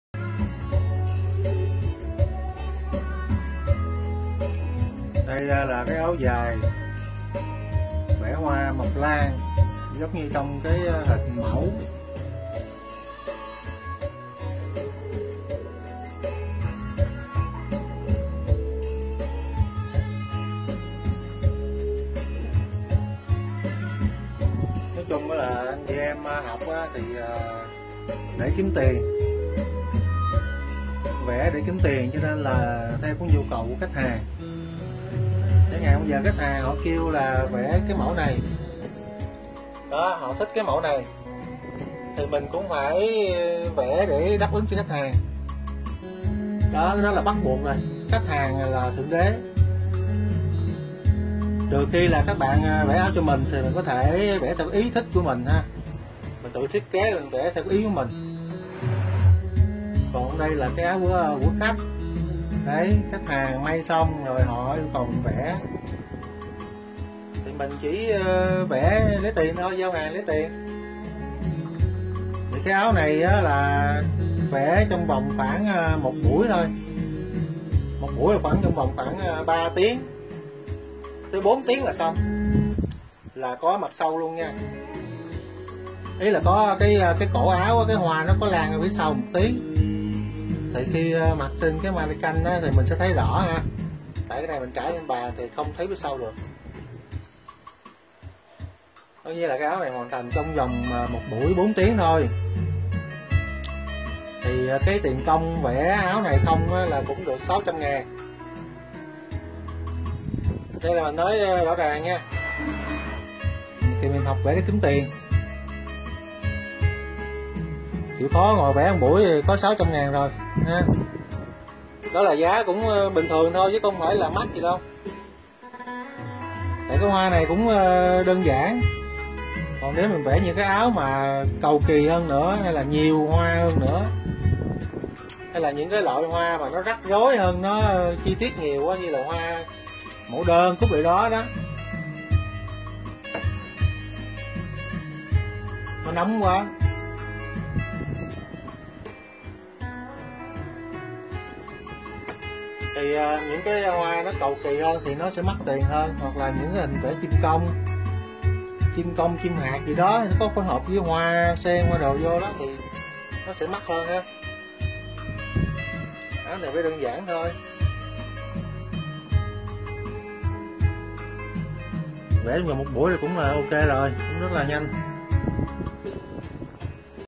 Dạy Vẽ Áo Dài hoa Mộc Lan sau 1 buổi tại lớp vẽ Viet Art - 0902.732.572
Học vẽ trên Vải - Áo dài - Đầm - Túi xách vải xem trang Facebook.com/dayveaodai hoặc Đt : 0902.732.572 gặp Thầy Việt